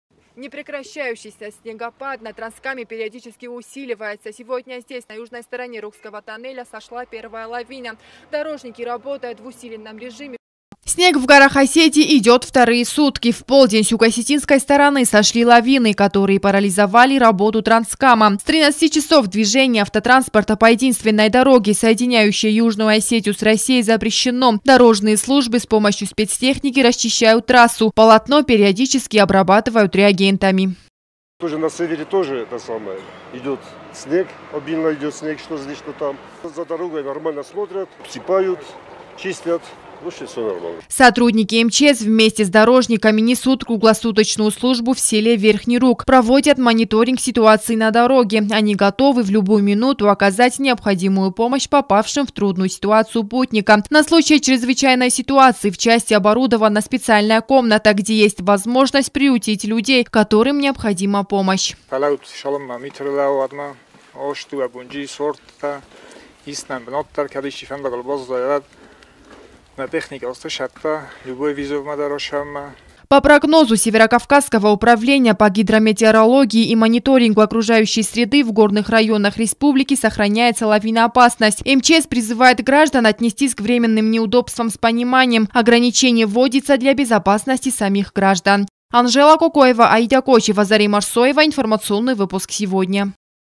Обильный снегопад и вероятность схода лавин. С 13-00 движение по траснкаму запрещено!
Сегодня с полудня движение по Транскавказской автомагистрали запрещено для всех видов транспорта. В течение нескольких дней сохраняется плохая погода. Не прекращается обильный снегопад в горах Осетии, как на северном портале Рокского тоннеля, так и на южном. Для обеспечения безопасности участников дорожного движения - Транскам будет закрыт для проезда до особого распоряжения.